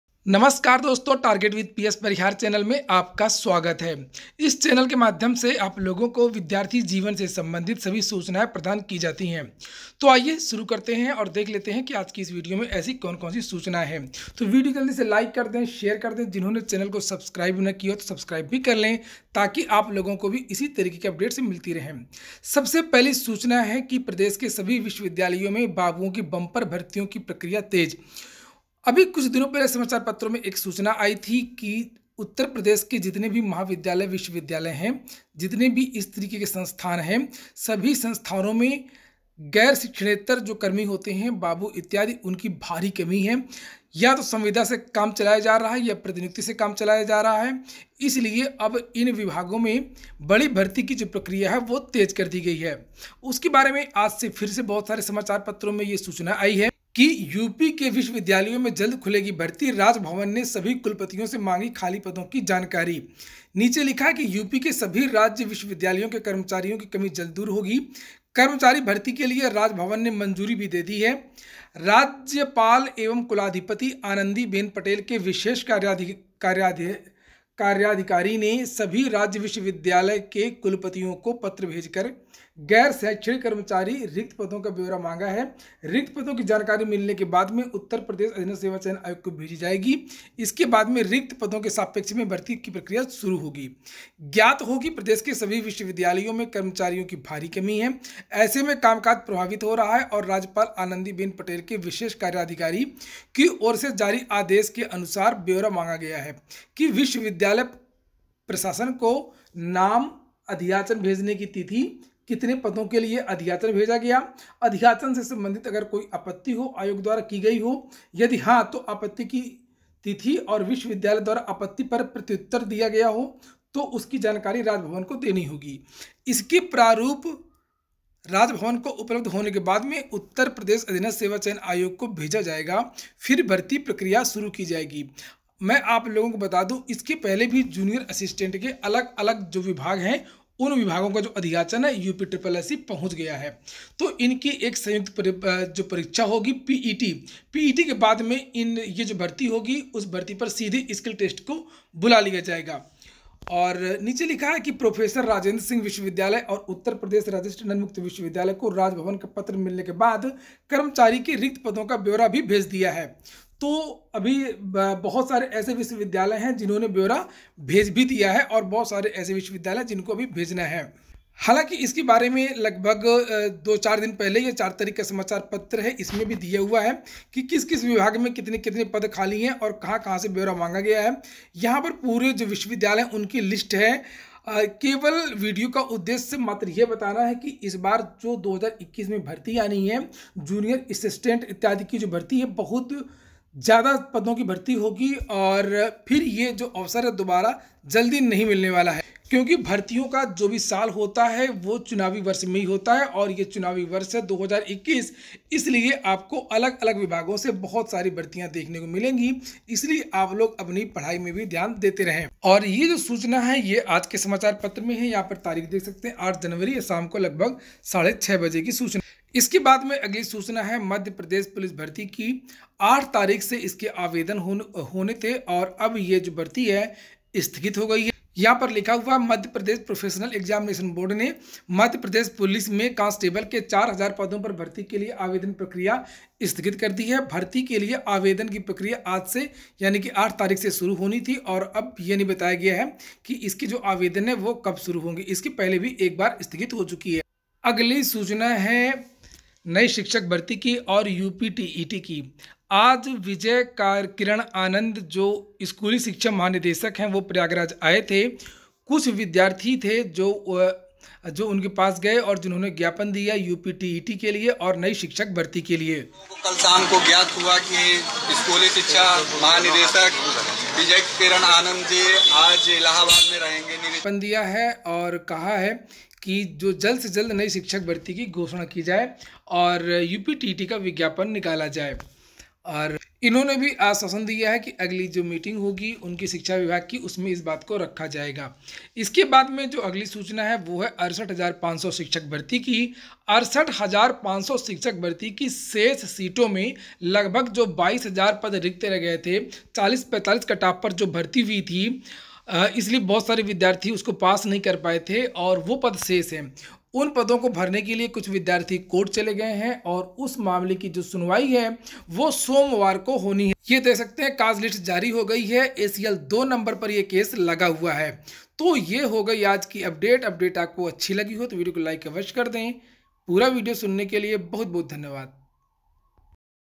सभी विश्वविद्यालयो मे बाबुओं की भर्ती प्रक्रिया तेज: uptet new teacher vacancy mp police constable
इस वीडियो में विभिन्न भर्तियों के बारे में महत्वपूर्ण जानकारी दी गई है